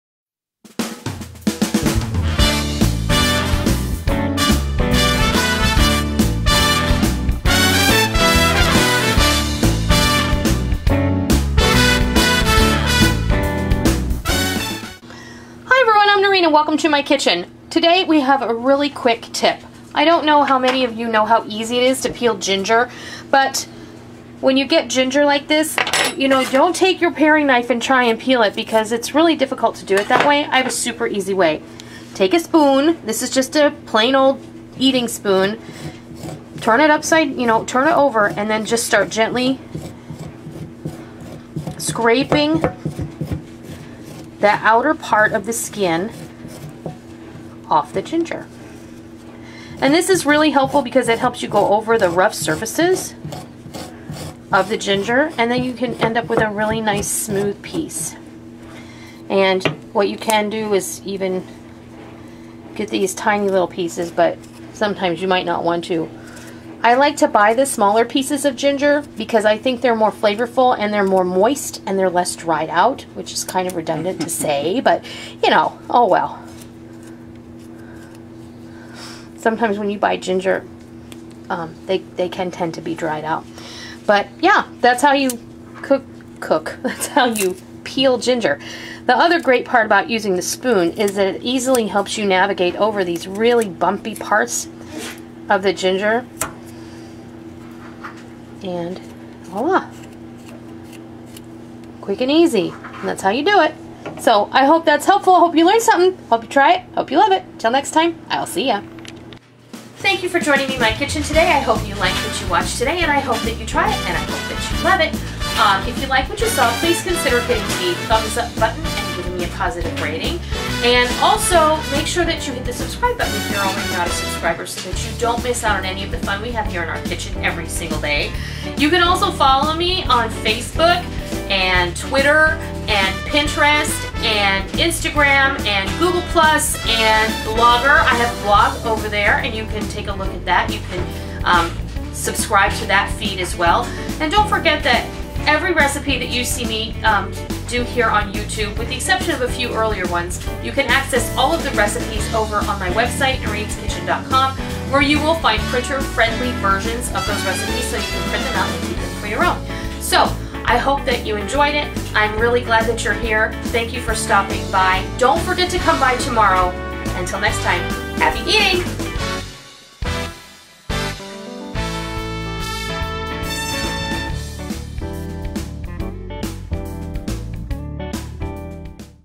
How to Peel Ginger Easily Noreen's Kitchen Quickie
Greetings!  I'm getting ready to make some delicious salmon with a soy ginger glaze.  So I need to peel some fresh ginger.  Did you know how easy it to peel ginger?  I'm going to show you!  Get your spoons and join me!

I hope you try this and I hope you love it!

Happy Eating!

***New Cookbook!
Cocoas & Cookies & More Galore!!

Don't forget Noreen's Kitchen Holiday Helper!